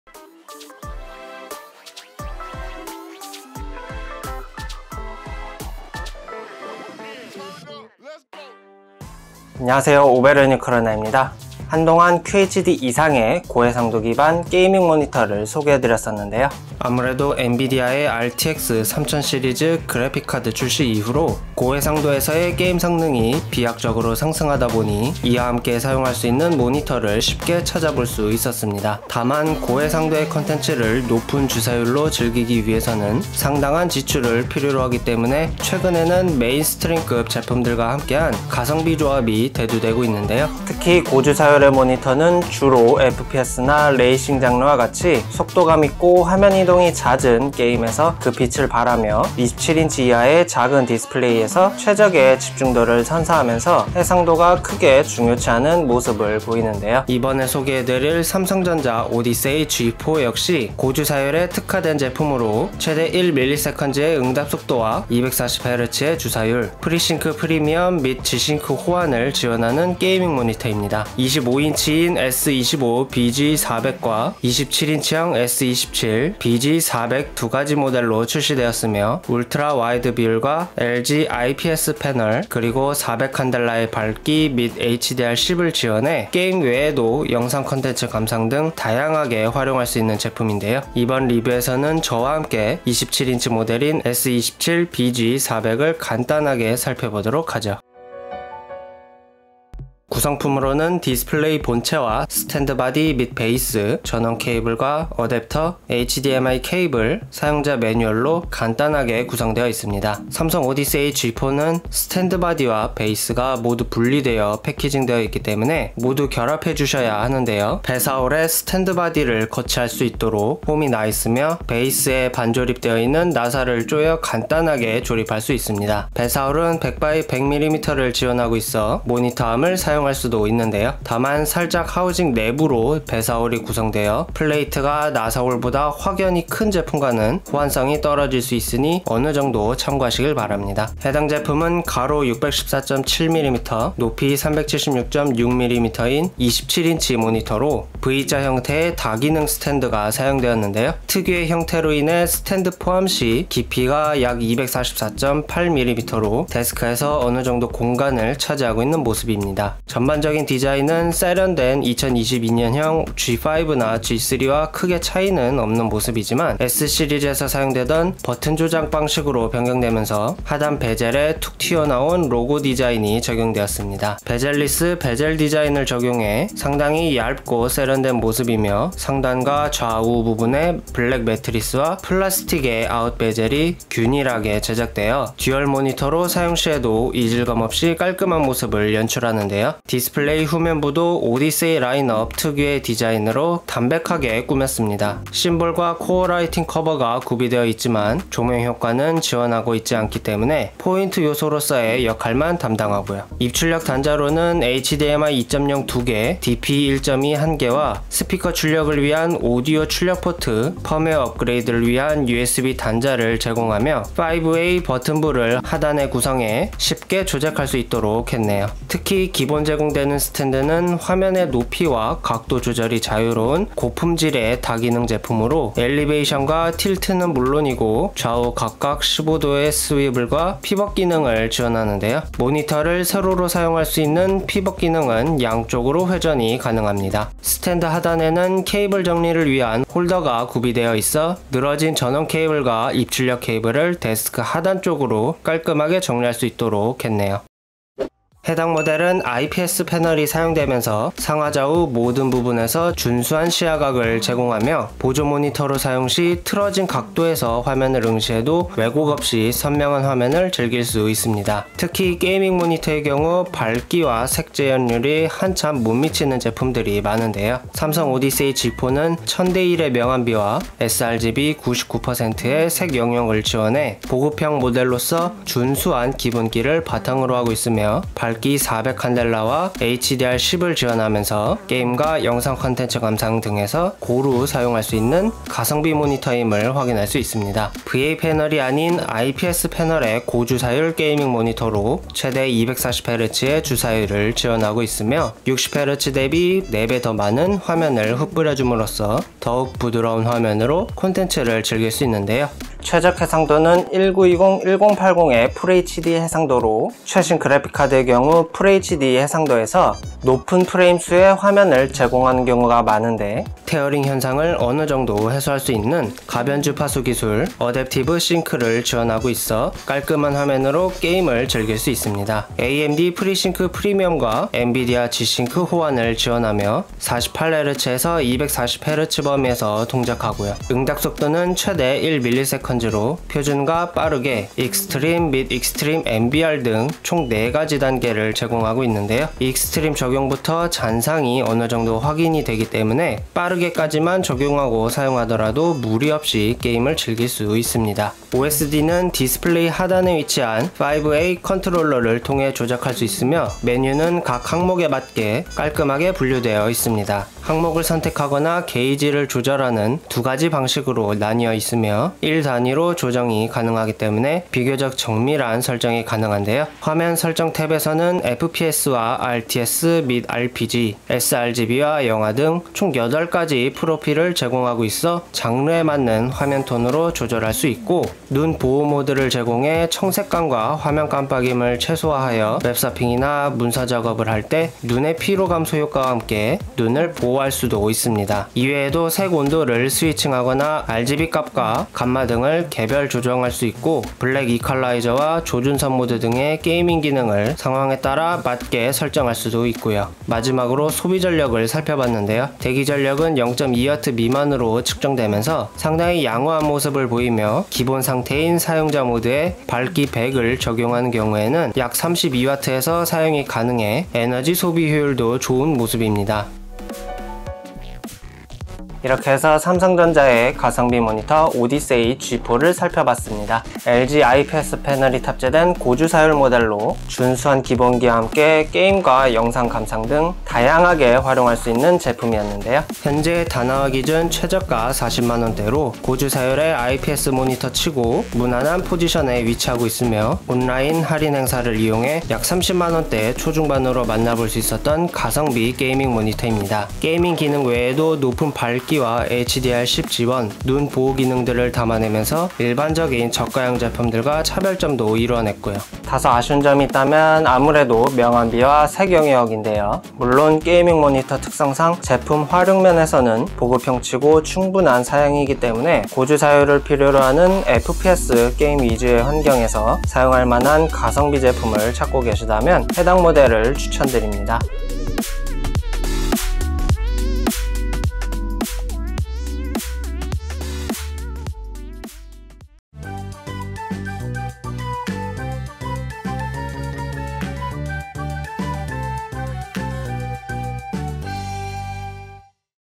240Hz 고주사율 LG IPS 패널에 준수한 기본기, 삼성전자 오디세이 G4 S27BG400
240Hz 고주사율 LG IPS 패널 + 준수한 기본기 + 다양한 활용 (게이밍 & 영화) = 삼성전자 오디세이 G4 S27BG400

다나와에서 무료 대여/ 제작비 지원

▶ 가격정보
삼성전자 오디세이 G4 S27BG400

제작: 오베르뉴 크로나
제공: 다나와 공식채널